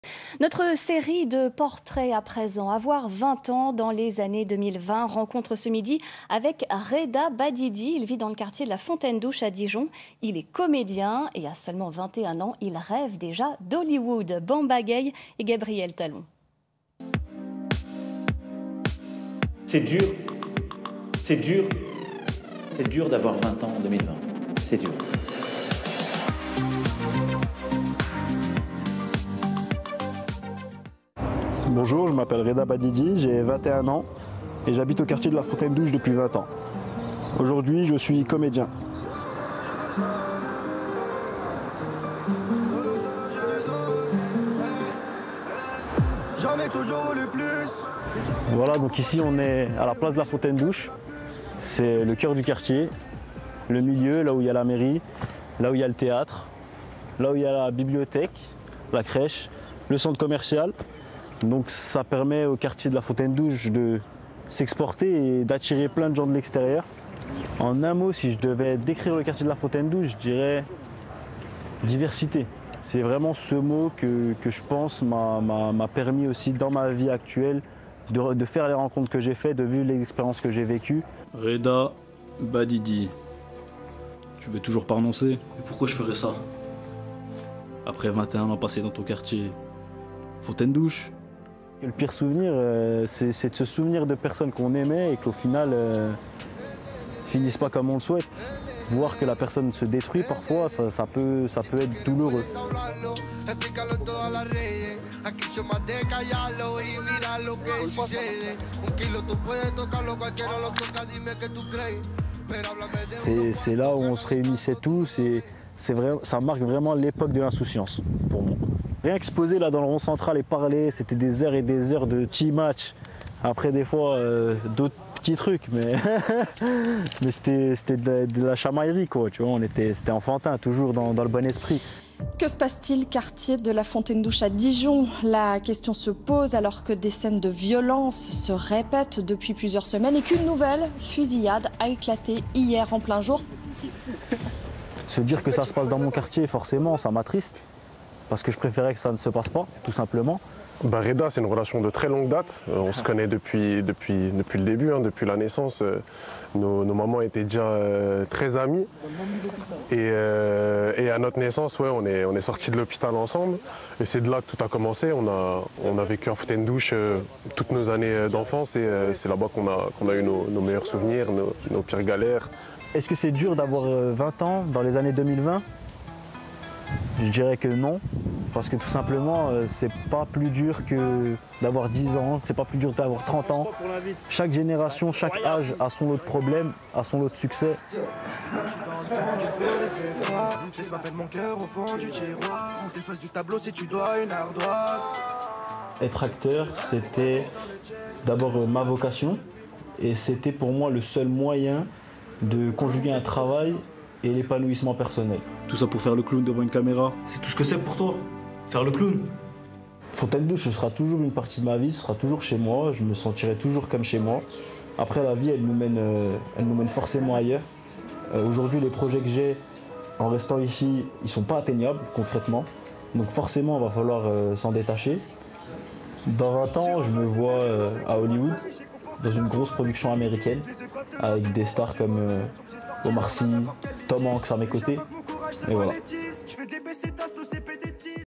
Avoir20ans : Reda, jeune de la Fontaine d'Ouche à Dijon
France 3 Bourgogne vous propose des portraits de jeunes qui ont 20 ans en 2021. Rencontre avec Reda Badidi. Il vit à la Fontaine d'Ouche et souhaite devenir comédien.
Reportage de Bamba Gueye et Gabriel Talon.